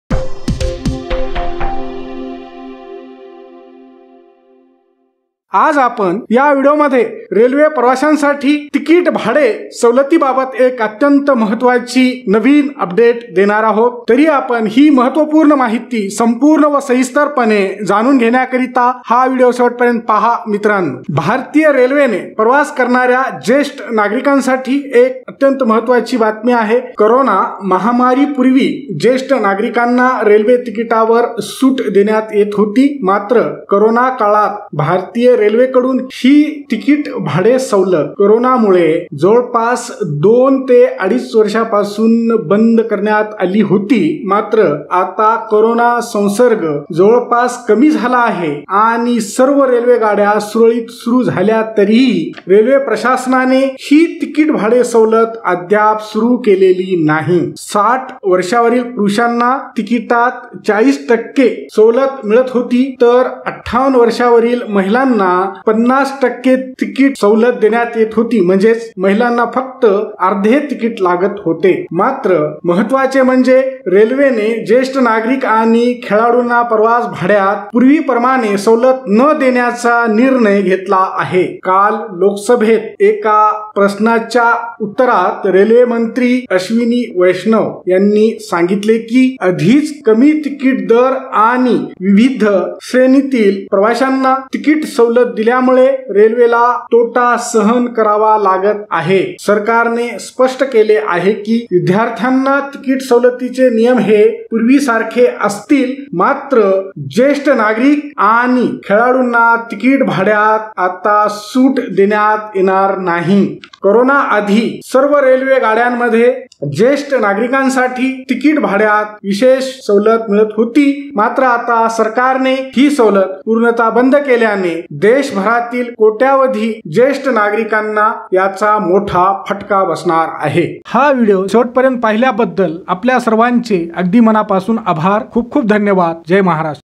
महत्त्वाची बातमी!रेल्वे प्रवाशांना तिकिटात सवलत..| railway senior citizen concession.
मित्रांनो आज या व्हिडिओ मध्ये  रेल्वे प्रवाशांना आता तिकिटात. | railway senior citizen concession. बद्दल महत्वाची माहीती देणार आहोत तरीही मित्रांनो हा व्हिडियो शेवट पर्यंत पहा.

धन्यवाद...